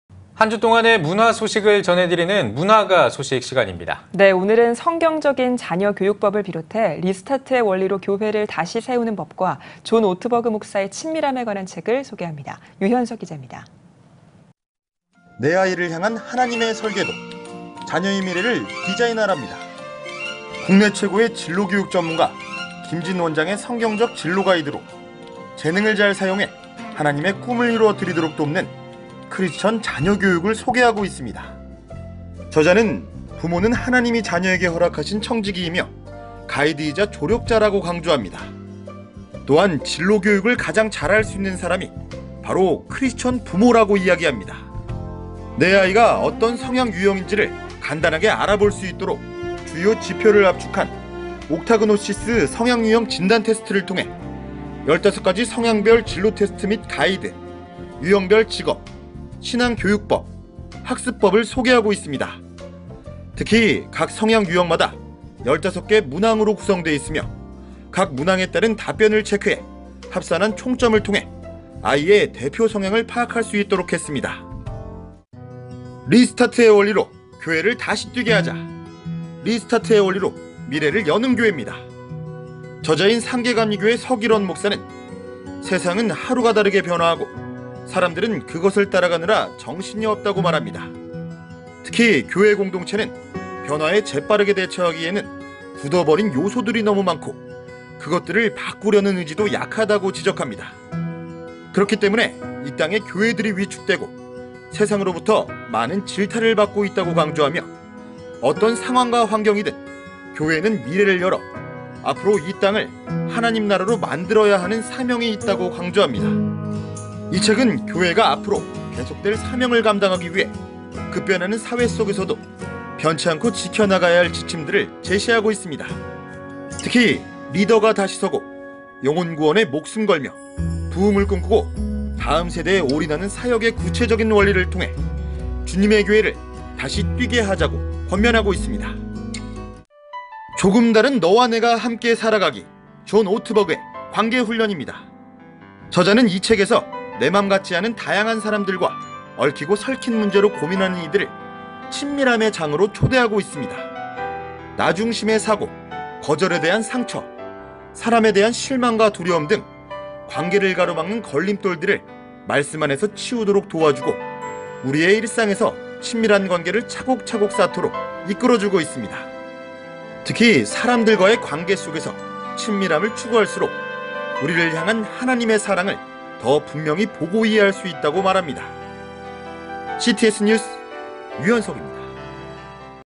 문화가 소식 – 책으로 여는 신앙 "자녀의 미래를 디자인하라", "리스타트의 원리로 미래를 여는 교회", "관계 훈련" (CTS뉴스, 기독교방송, 기독뉴스, 기독교뉴스)
문화가 소식입니다. 오늘은 성경적으로 자녀를 키우는 자녀 교육법을 비롯해 리스타트의 원리로 교회를 다시 세우는 법과 존 오트버그 목사의 친밀함에 관한 책을 소개합니다. 유현석 기잡니다.

- 내 아이를 향한 하나님의 설계도, "자녀의 미래를 디자인하라"입니다.
국내 최고의 진로교육전문가 김진 원장의 성경적 진로 가이드로, 재능을 잘 사용해 하나님의 꿈을 이루어 드리도록 돕는 크리스천 자녀 교육을 소개하고 있습니다.
저자는 부모는 하나님이 자녀에게 허락하신 청지기이며, 가이드이자 조력자라고 강조합니다. 또한 진로 교육을 가장 잘할 수 있는 사람이 바로 크리스천 부모라고 이야기합니다.
내 아이가 어떤 성향유형인지를 간단하게 알아볼 수 있도록 주요 지표를 압축한 ‘옥타그노시스(OCTAGNOSIS)’성향유형 진단 테스트를 통해 15가지 성향별 진로 테스트 및 가이드, 유형별 직업, 신앙교육법, 학습법을 소개하고 있습니다.
특히, 각 성향유형마다 15개 문항으로 구성돼 있으며, 각 문항에 따른 답변을 체크해 합산한 총점을 통해 아이의 대표 성향을 파악할 수 있도록 했습니다 .

- 리스타트의 원리로 교회를 다시 뛰게 하자!, "리스타트의 원리로 미래를 여는 교회"입니다.
저자인 상계감리교회 서길원 목사는“세상은 하루가 다르게 변화하고, 사람들은 그것을 따라가느라 정신이 없다”고 말합니다.
특히, 교회 공동체는 변화에 재빠르게 대처하기에는 굳어 버린 요소들이 너무 많고, 그것들을 바꾸려는 의지도 약하다고 지적합니다.
그렇기 때문에 이 땅의 교회들이 위축되고, 세상으로부터 많은 질타를 받고 있다고 강조하며, 어떤 상황과 환경이든 교회는 미래를 열어 앞으로의 이 땅을 하나님 나라로 만들어야 하는 사명이 있다고 강조합니다.
이 책은 교회가 앞으로 계속될 사명을 감당하기 위해 급변하는 사회 속에서도 변치 않고 지켜 나가야 할 지침들을 제시하고 있습니다.
특히, 리더가 다시 서고, 영혼 구원에 목숨 걸며 부흥을 꿈꾸고 다음 세대에 올인하는 사역의 구체적인 원리를 통해 주님의 교회를 다시 뛰게 하자고 권면하고 있습니다.

- 조금 다른 너와 내가 함께 살아가기, 존 오트버그의 "관계훈련"입니다.
저자는 이 책에서 내 맘 같지 않은 다양한 사람들과 얽히고 설킨 문제로 고민하는 이들을 친밀함의 장으로 초대하고 있습니다.
나 중심의 사고, 거절에 대한 상처, 사람에 대한 실망과 두려움 등 관계를 가로막는 걸림돌들을 말씀 안에서 치우도록 도와주고, 우리의 일상에서 친밀한 관계를 차곡차곡 쌓도록 이끌어주고 있습니다. 특히, 사람들과의 관계 속에서 친밀함을 추구할수록 우리를 향한 하나님의 사랑을 더 분명히 보고 이해할 수 있다고 말합니다. CTS뉴스 유현석입니다.